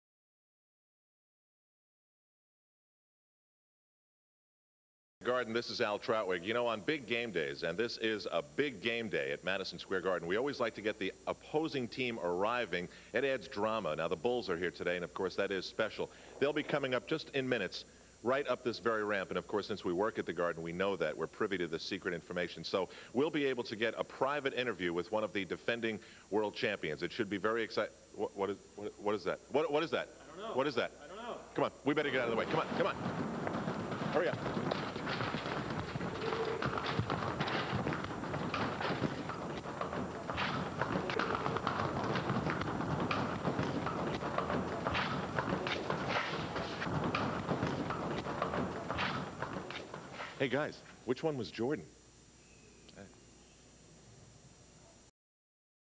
VERY RARE: Michael Jordan (Age 29) Pre-Game Interview w/Al Trautwig (1992)
November 28, 1992:
Very rare Michael Jordan pre-game feature with Al Trautwig of then MSG Network.